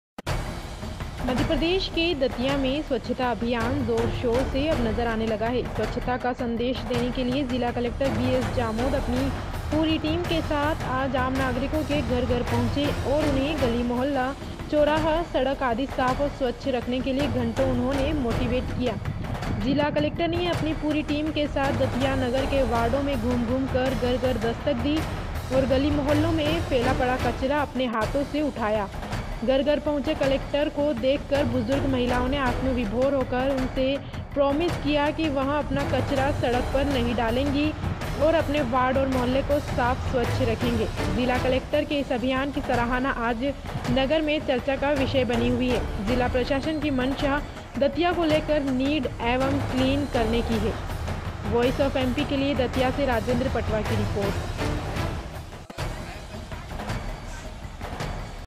स्‍वच्‍छता अभियान को लेकर दतिया कलेक्‍टर उतरे मैदान में, अपने हाथों से उठाया कचरा, हम करेगें क्‍लीन
मध्यप्रदेश के दतिया में स्वच्छता का अभियान जोर शोर से अब नजर आने लगा है। स्वच्छता का संदेश देने के लिए जिला कलेक्टर बी.एस. जामोद अपनी पूरी टीम के साथ आज आम नागरिकों के घर घर पहुंचे और उन्हें गली मोहल्ला ,चौराहा , सड़क आदि साफ और स्वच्छ रखने के लिए घंटों उन्होंने मोटिवेट किया। जिला कलेक्टर ने अपनी पूरी टीम के साथ दतिया नगर के वार्डों में घूम घूम कर घर-घर दस्तक दी और गली मोहल्लों में फैला पड़ा कचरा अपने हाथों से उठाया। घर घर पहुंचे कलेक्टर को देखकर बुजुर्ग महिलाओं ने आत्म विभोर होकर उनसे प्रॉमिस किया कि वह अपना कचरा सड़क पर नहीं डालेगी और अपने वार्ड और मोहल्ले को साफ स्वच्छ रखेंगे। जिला कलेक्टर के इस अभियान की सराहना आज नगर में चर्चा का विषय बनी हुई है। जिला प्रशासन की मंशा दतिया को  लेकर  नीट एंड क्लीन बनाने की है।
दतिया से राजेन्‍द्र पटवा की रिपोर्ट